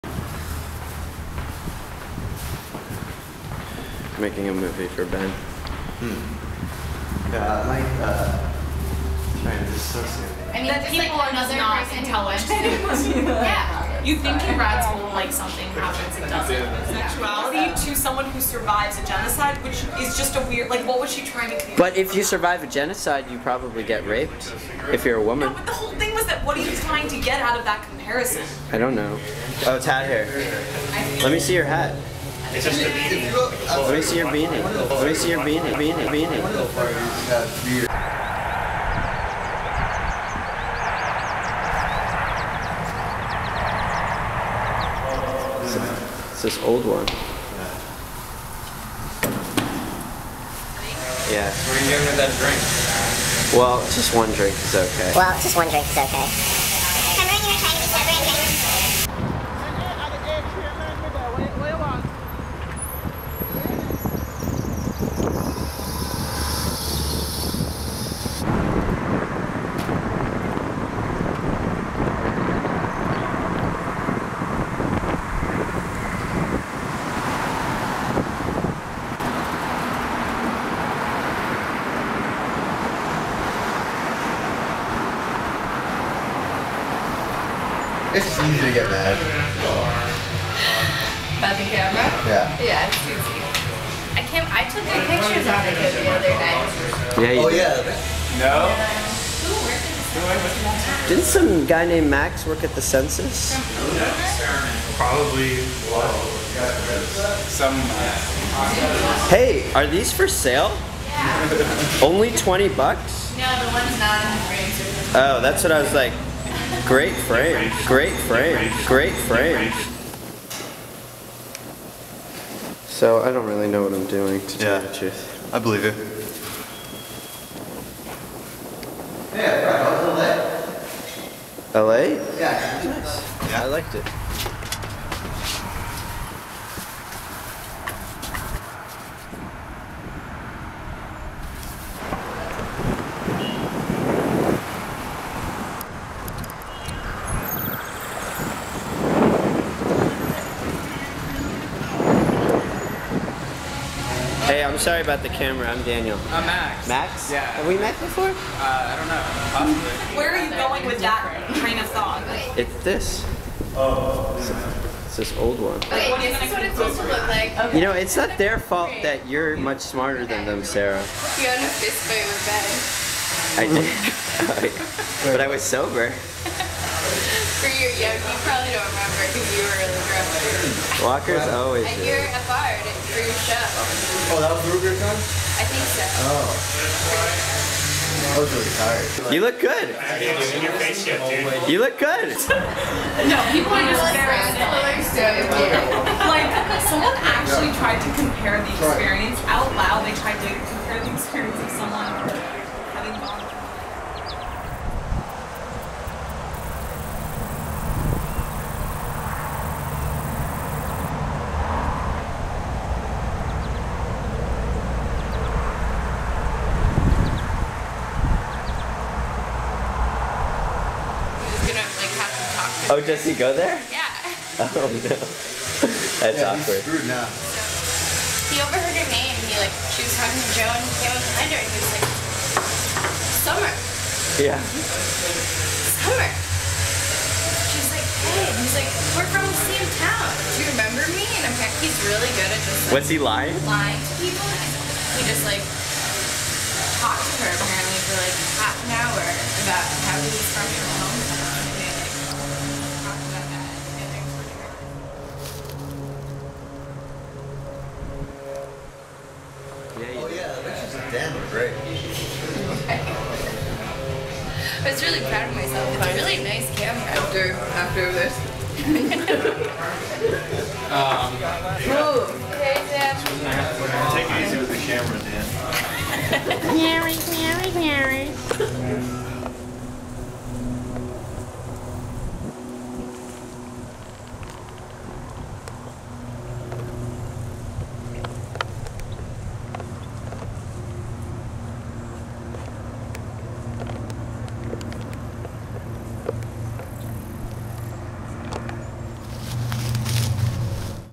Project Landswet Tailcock Party
Footage recorded by Daniel Terna imposed on edit structure of The Wetlands Project video profile made by Oberlin College's Office of Communications